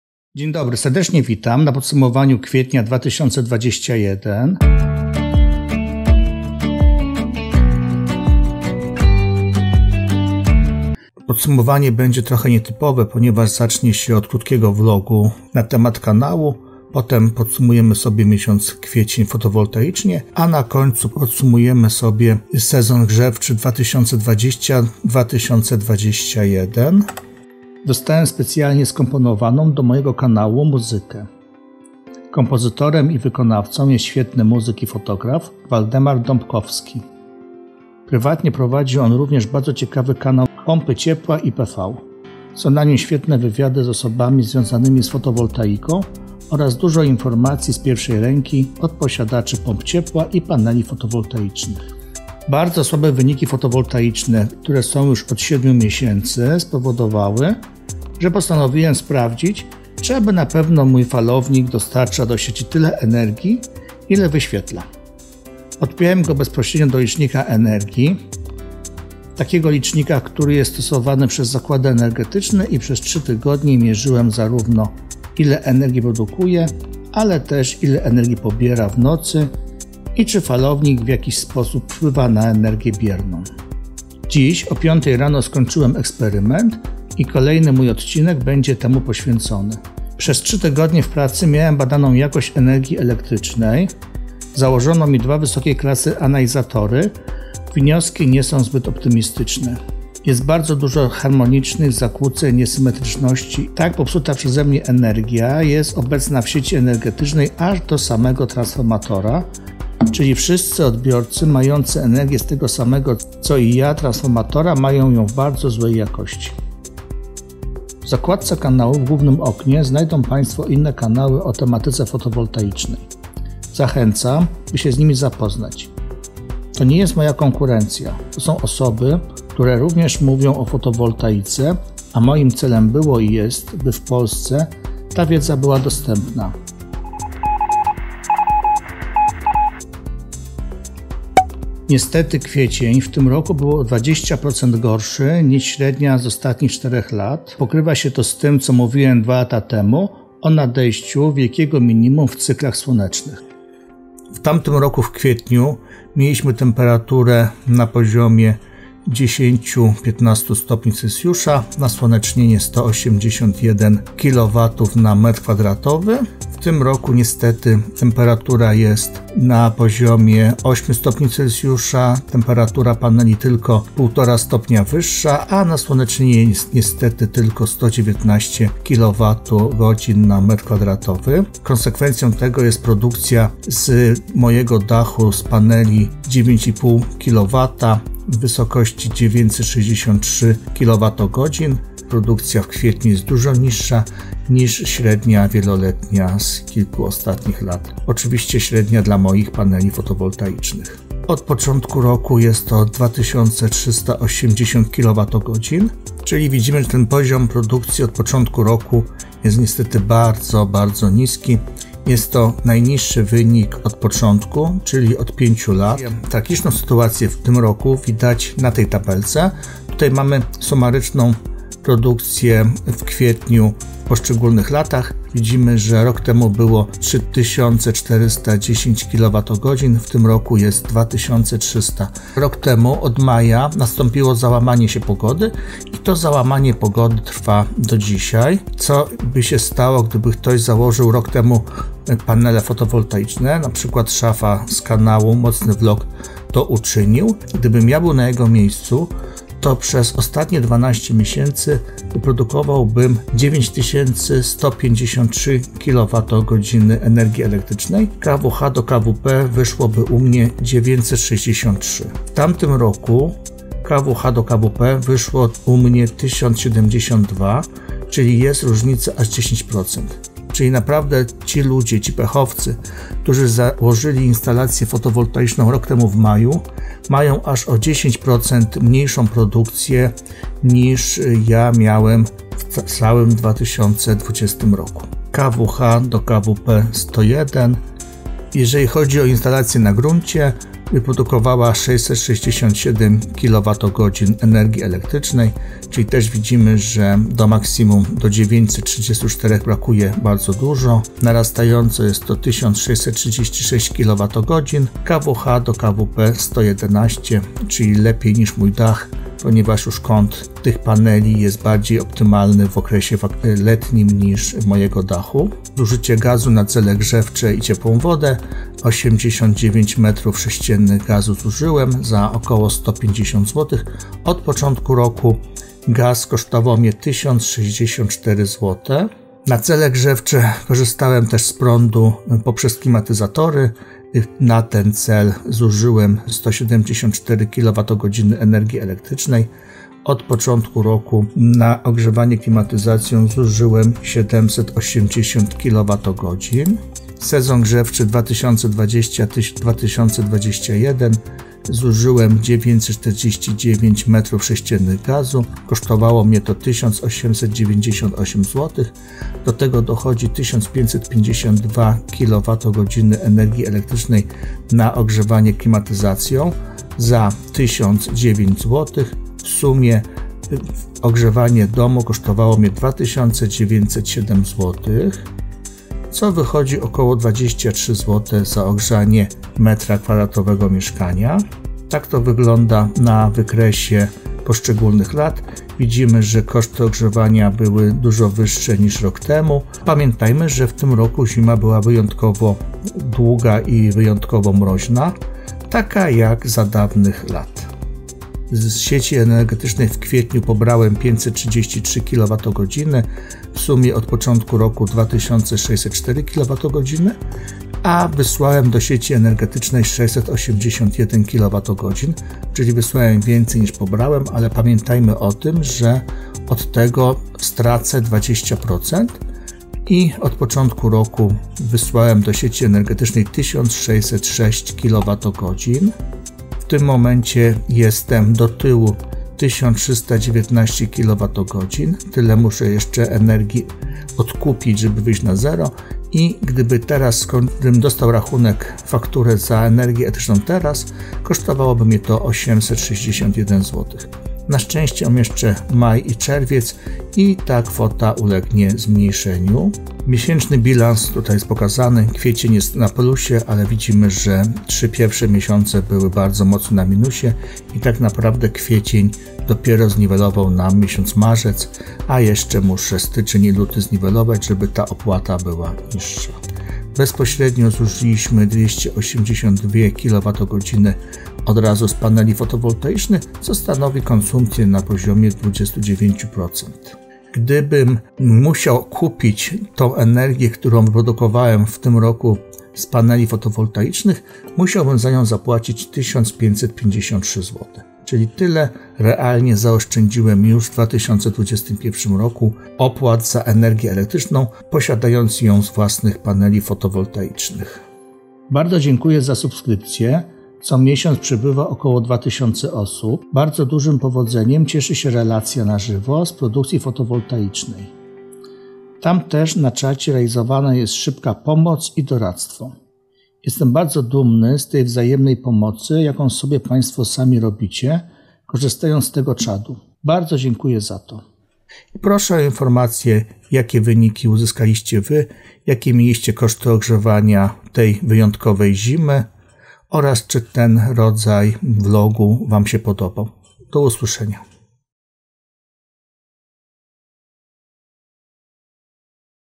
Podsumowanie Kwietnia i podsumowanie sezonu grzewczego
Pełne koszty ogrzewania gazem i prądem sezon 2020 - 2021
Podsumowanie kwietnia 2021
Martwisz się, że Twoja nowa instalacja fotowoltaiczna słabo dział?
To wina pogody.

Panele⚡Fotowoltaiczne
⚡FOTOWOLTAIKA
⚡Fotowoltaika⚡